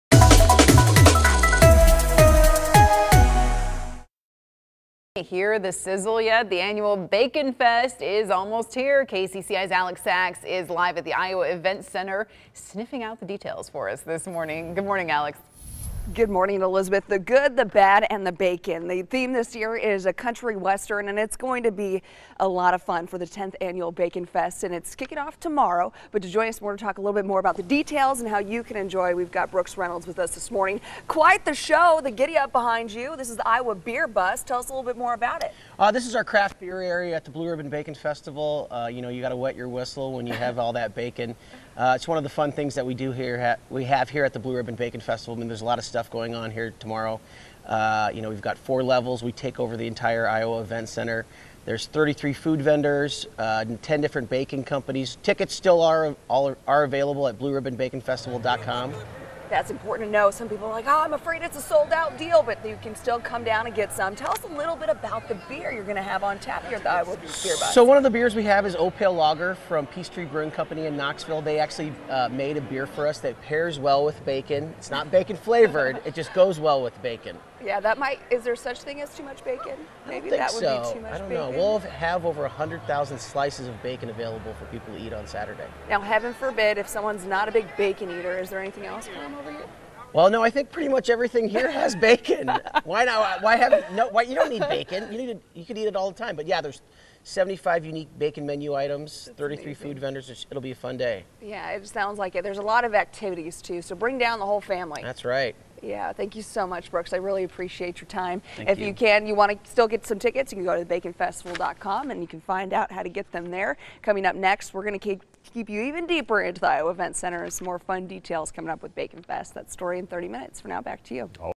Blue Ribbon Baconfest sizzles this weekend
The 10th annual Baconfest is ready to get underway at the Iowa Events Center.